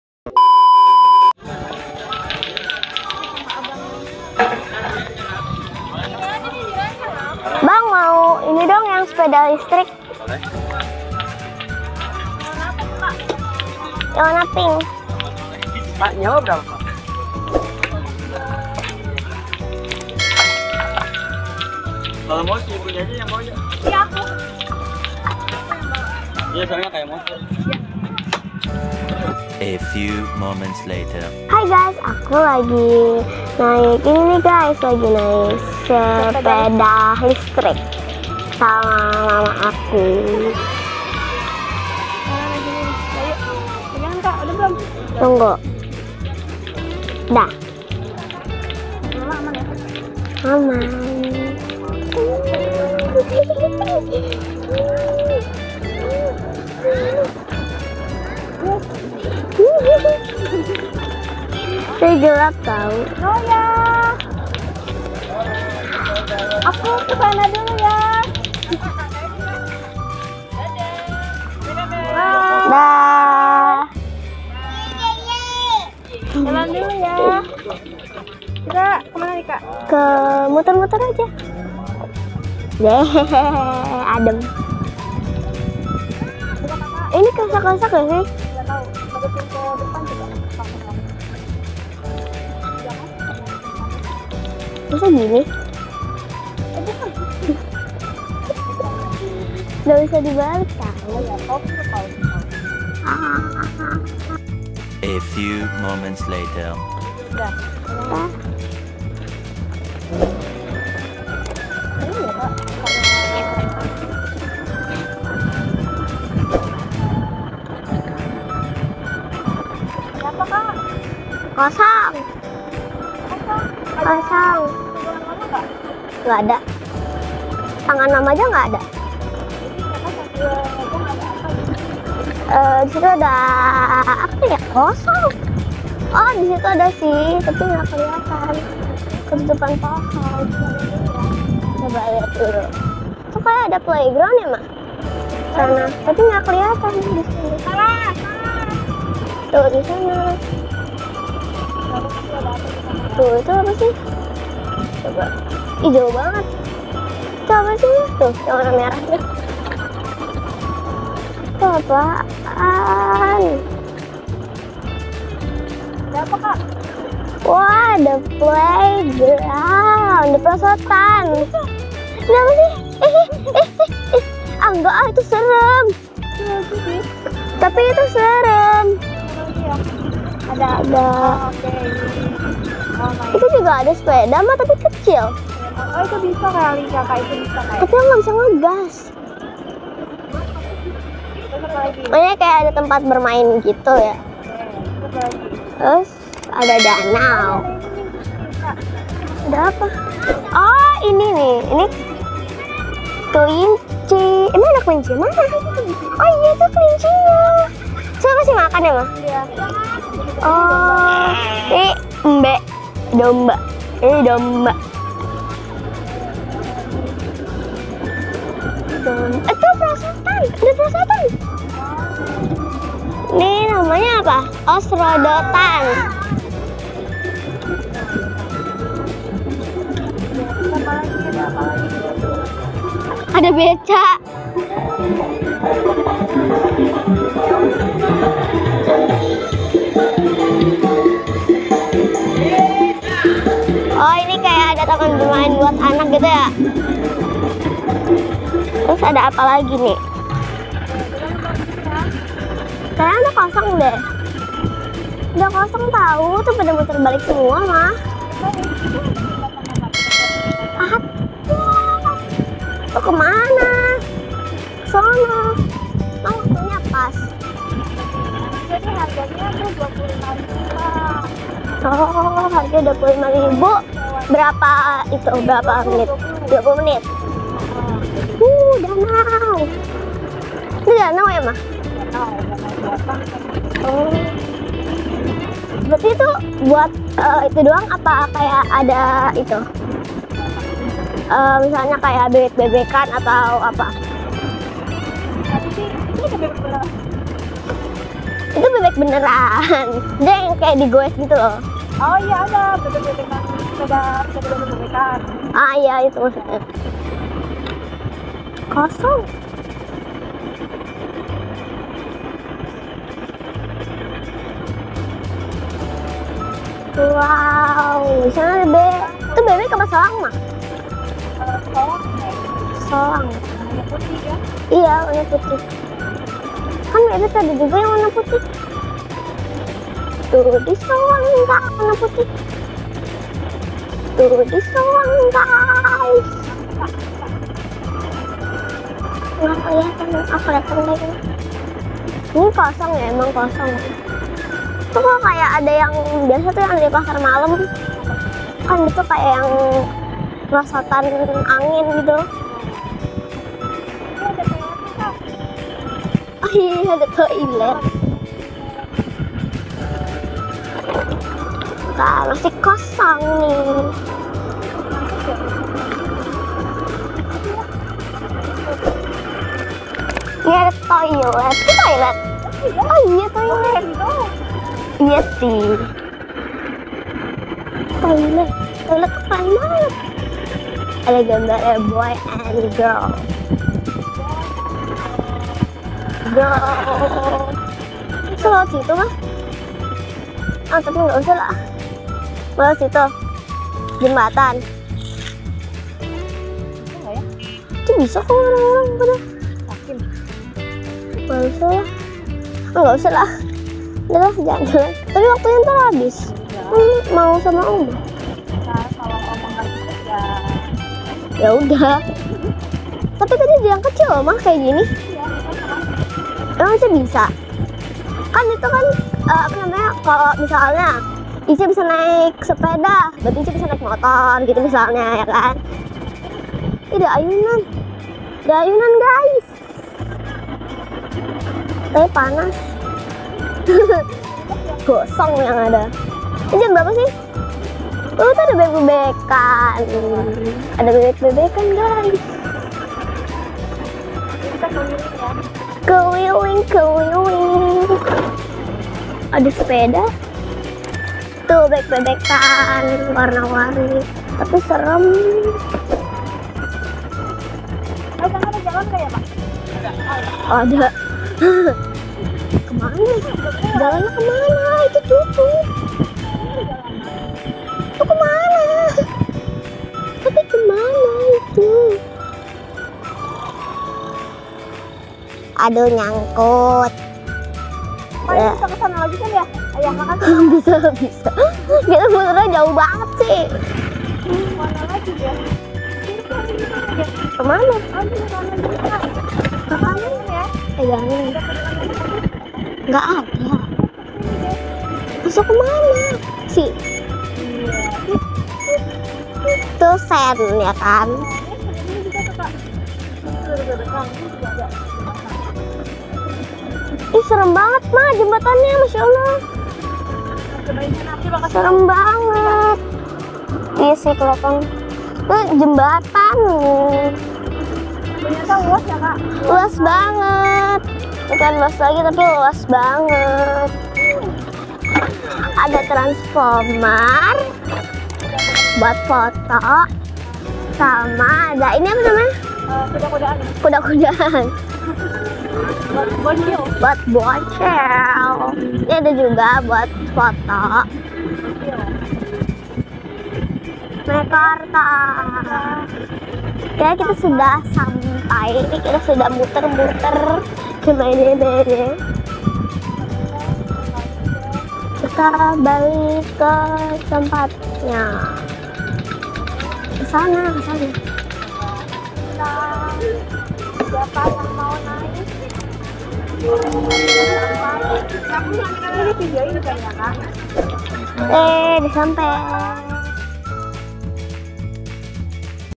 SEWA SEPEDA LISTRIK KELILING AREA CENTRAL PARK MEIKARTA | VLOG SERU
Hai Guys,

Ketemu lagi di Channel YouTube Raissa Anindya Putri I.C.H.A di video kali ini aku ada di Central Park Meikarta, di sini aku mau nyobain sepeda listrik tapi ternyata aku belum bisa, hehe..

Jadi yang bawa sepeda listrik nya mama aku, di sini aku full ngelilingin area central park Meikarta, pokoknya seru banget deh